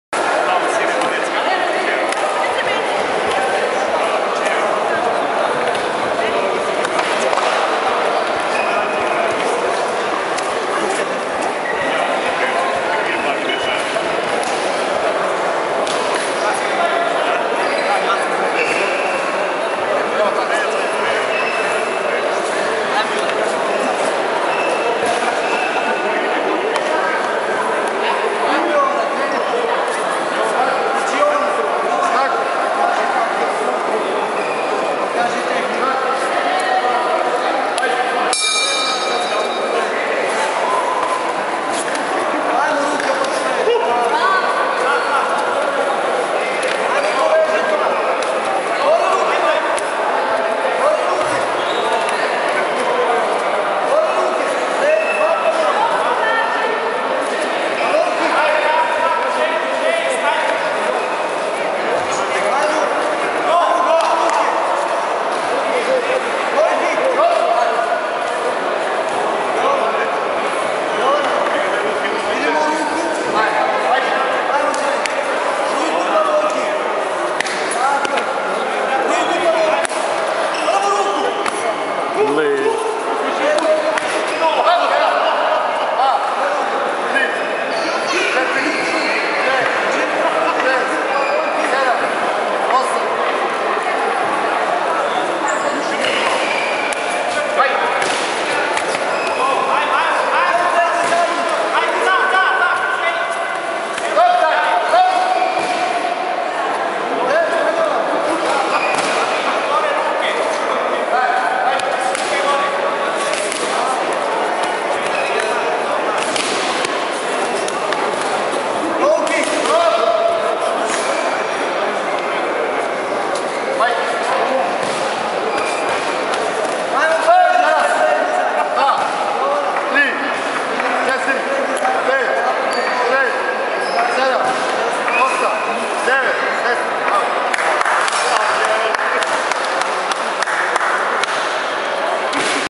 Jovana Ignjatovic - KBK Vlasotince
Prvenstvo Srbije, disciplina Low Kick, Kadeti kategorija do 55kg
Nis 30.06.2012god